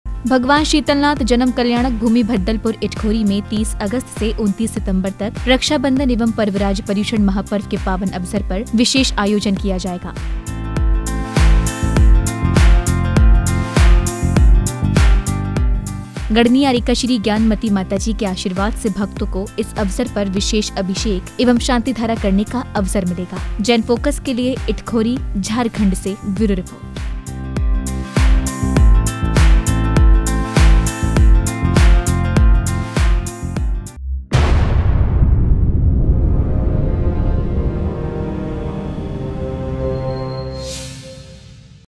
भद्दलपुर में होगा विशेष अभिषेक और शांतिधारा | Itkori News | JAIN FOCUS
- भद्दलपुर में होगा विशेष अभिषेक और शांतिधारा
- 30 अगस्त से 29 सितम्बर तक होगा आयोजन
- मनाया जाएगा रक्षाबंधन एवं पर्वराज पर्युषण महापर्व
- गणिनी आर्यिकाश्री ज्ञानमति माताजी का मिलेगा आशीर्वाद